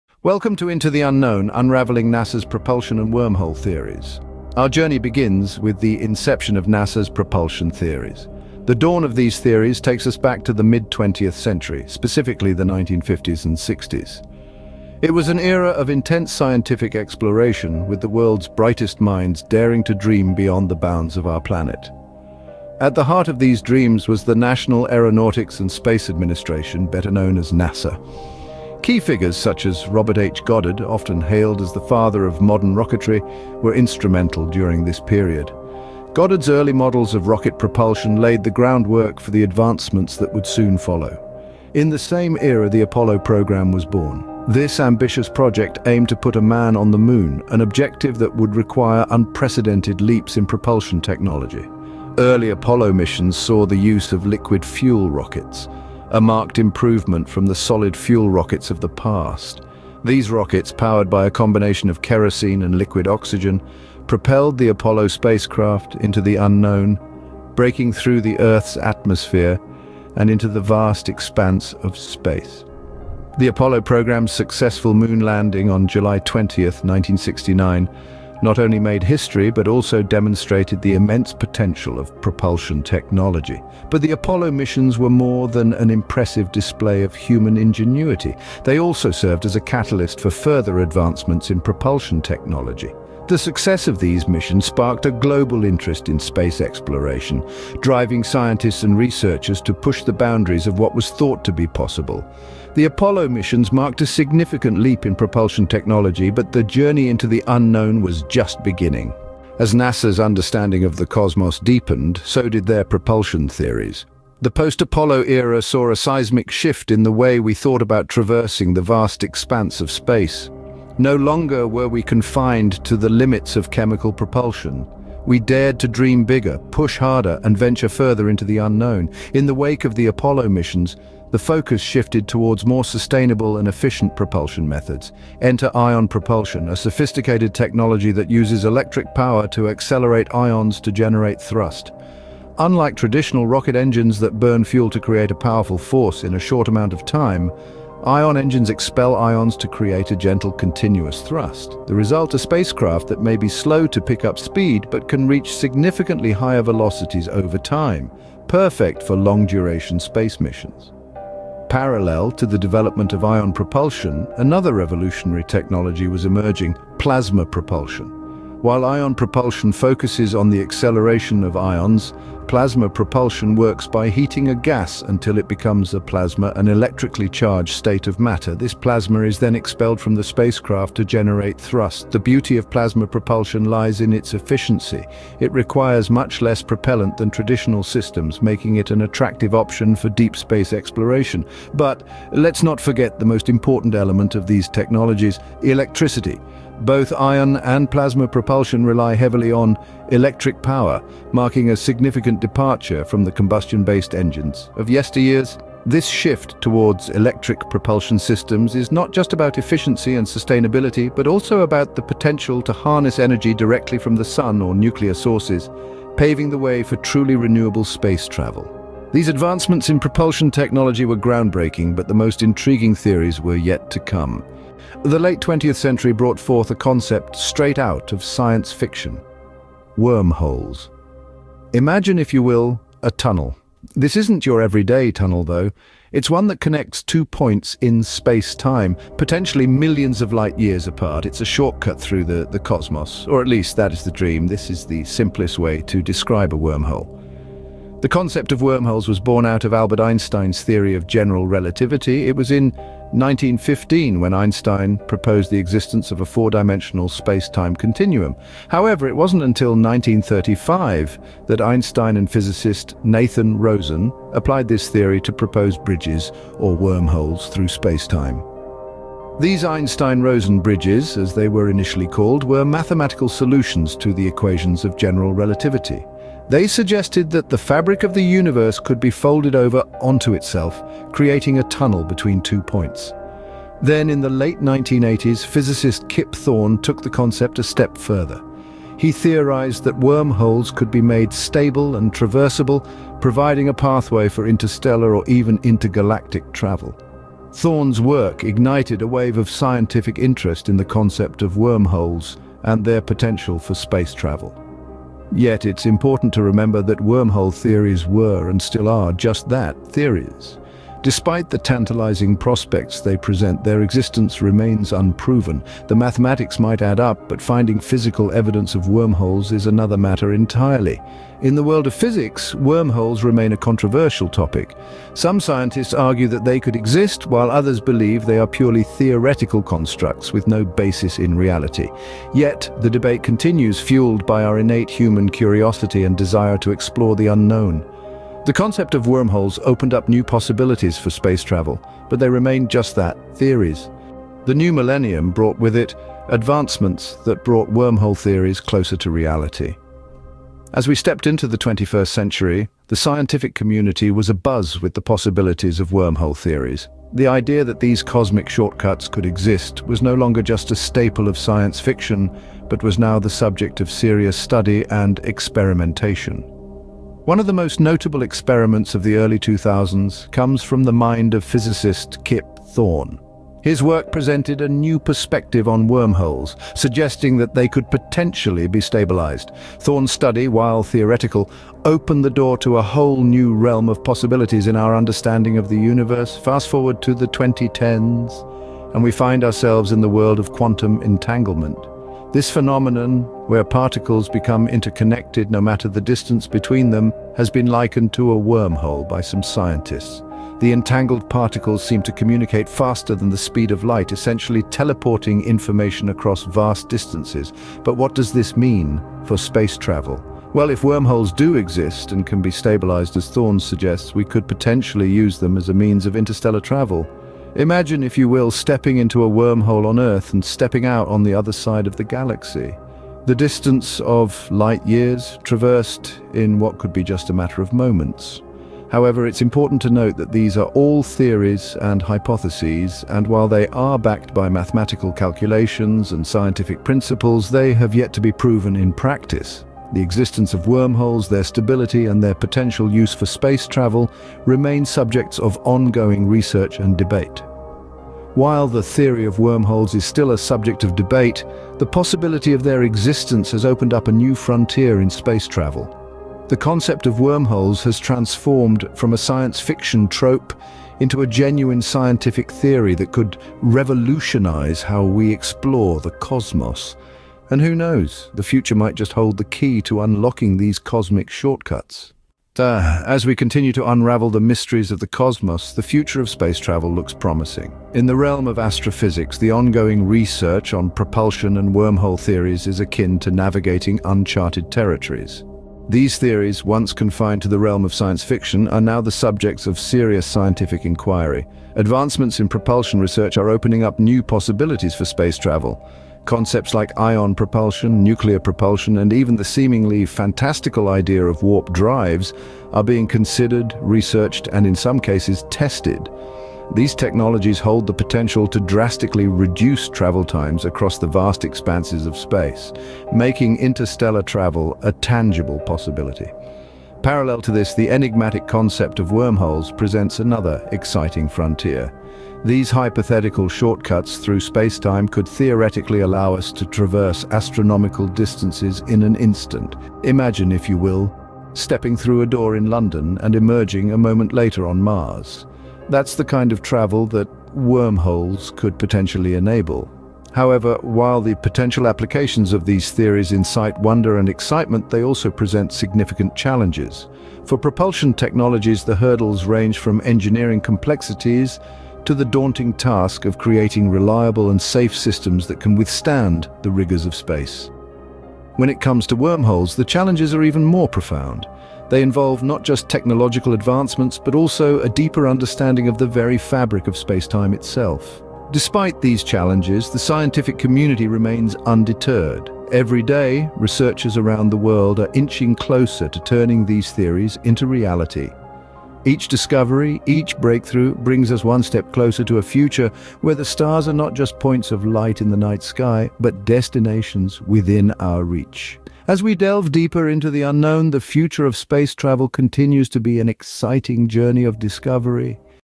Unraveling NASA's Propulsion & Wormhole Theories
Created By Steve Rinker

OUTLINE:
00:00:00
The Dawn of Propulsion Theories

00:01:49
The Evolution of Propulsion

00:03:44
The Birth of Wormhole Theories

00:05:44
The Reality of Wormholes

00:07:52
The Future of Space Travel

In this captivating video, we explore NASA's groundbreaking Propulsion and Wormhole Theories that have left scientists and enthusiasts awe-struck. Join us as we embark on a mesmerizing journey "Into the Unknown: Unraveling NASA's Propulsion & Wormhole Theories." Prepare to be enthralled by the mind-boggling advancements that NASA has made in the realm of propulsion technology. We'll uncover the secrets behind their cutting-edge propulsion systems that could potentially revolutionize space travel as we know it. From ion drives to plasma thrusters, NASA's scientists have pushed the boundaries of imagination and engineering to develop faster, more efficient means of traversing the cosmos. 🌌

But that's not all – get ready to have your mind blown as we delve into the mind-bending concept of wormholes. NASA has long been intrigued by the possibility of using these hypothetical tunnels through spacetime for interstellar travel. Our experts will guide you through the mind-boggling theories and ongoing research surrounding wormholes, presenting the latest discoveries while pondering the potential implications for the future of space exploration. 🌠

As we unravel the mysteries behind NASA's propulsion and wormhole theories, we'll also showcase awe-inspiring visuals captured by space probes, telescopes, and astronauts. 🛰️🔭👨‍🚀 These stunning images will transport you to the outer reaches of the universe, instilling a sense of wonder and curiosity about our place in the cosmos. Join us on this extraordinary adventure as we strive to understand the mind-blowing concepts that NASA is actively exploring. From propulsion advancements to mind-bending theories of wormholes, we'll broaden your horizons and leave you with profound new perspectives on the vastness of space and the limitless possibilities that lie beyond. Don't miss out on this captivating journey "Into the Unknown: Unraveling NASA's Propulsion & Wormhole Theories." Together, let's ignite our passion for space exploration and unlock the secrets of the universe! 🌌🚀🌠